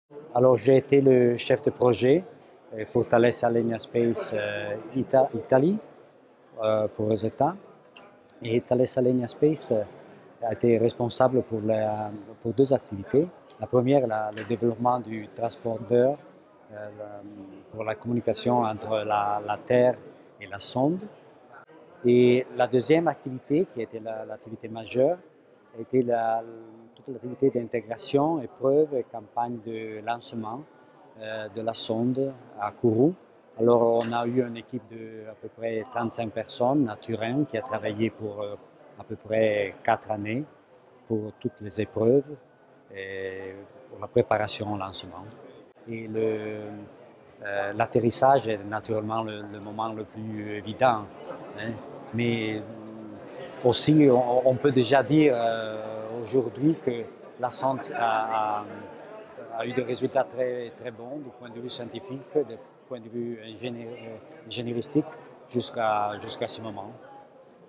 Paolo Musi - Thales Alenia Space et Rosetta
Thales Alenia Space est un sous-contractant sur la mission Rosetta et Philae (Airbus Defence and Space est l'industriel principal). Paolo Musi, chef de projet Rosetta chez Thales Alenia Space nous explique le rôle de cet industriel spatial.